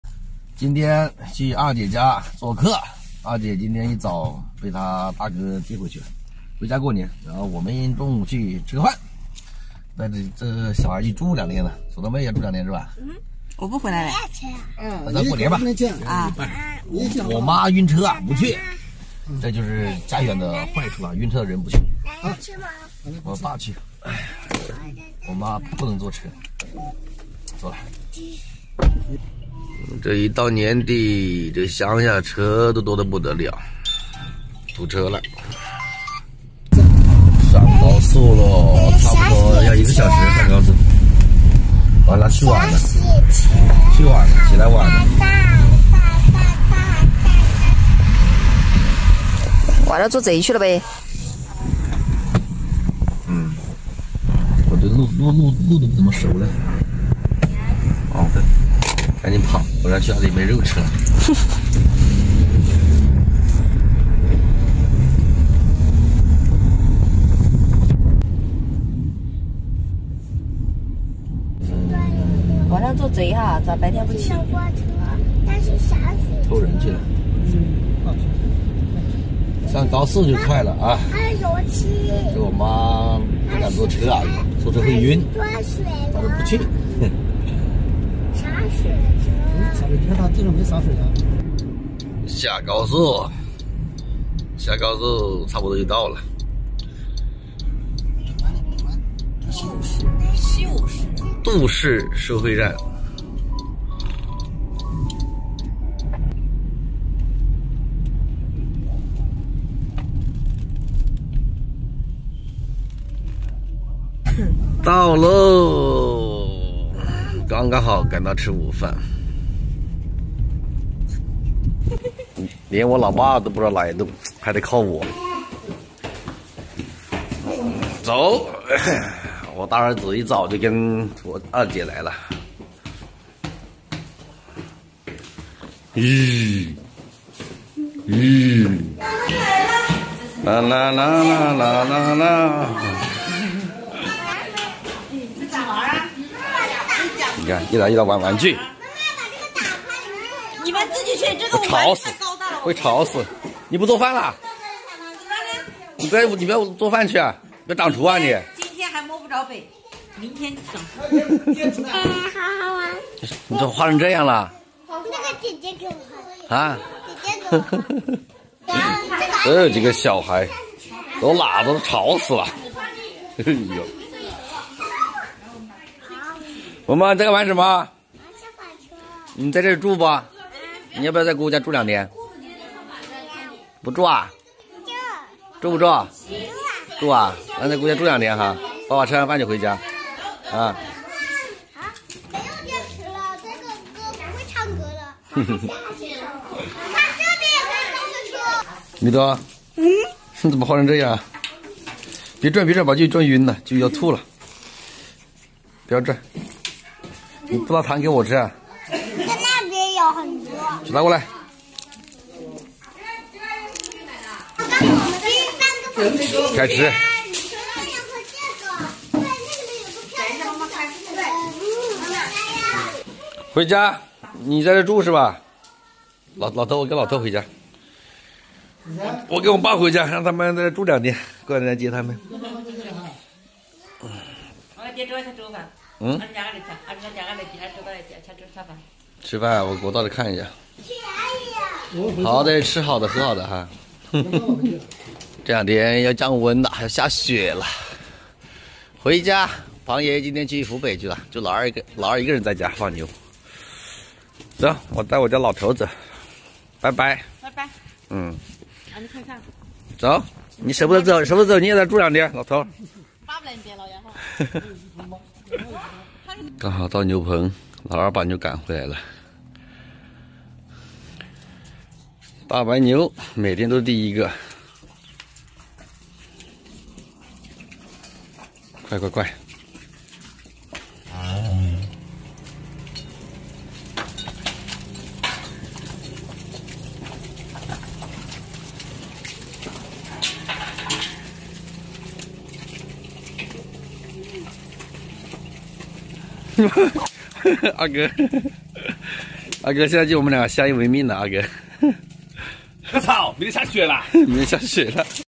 去二姐家做客，老媽不願意去，媳婦不願意回，這是什麼情況【石頭秀】
去二姐家做客，土豆妹要留在那裡過年，石頭和老爸先回家了